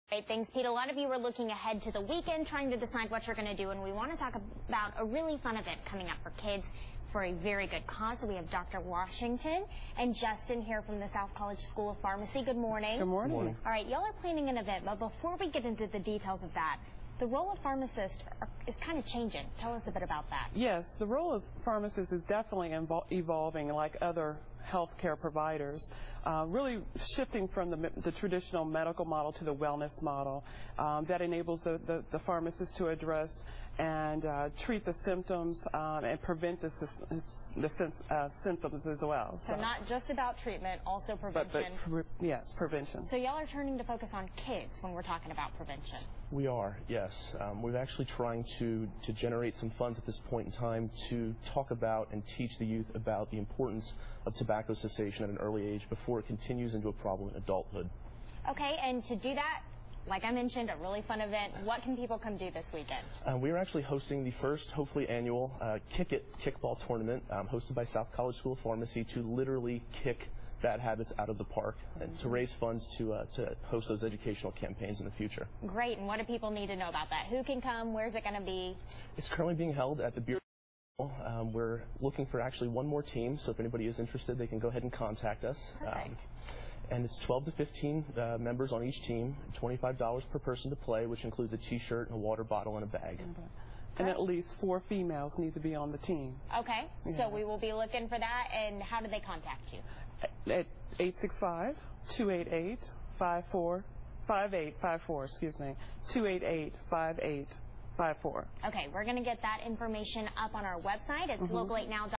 South College School of Pharmacy launches Kick-it program
The South College School of Pharmacy is launching a new educational program to help adolescents kick bad habits. Watch this clip from WVLT-TV Channel 8 to learn more.